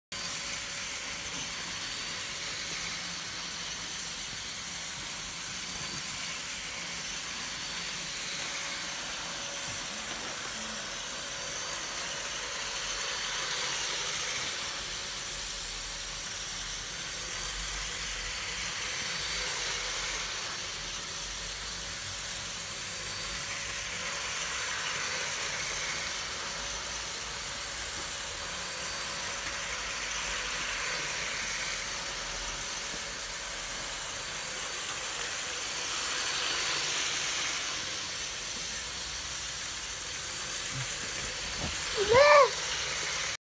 Thomas the Tank Engine’s First Proper Run
Hornby OO Gauge Thomas the Tank Engine having his first proper run in the lounge, now that I’ve finally fixed his con-Rod. Cue - one very happy four year old boy!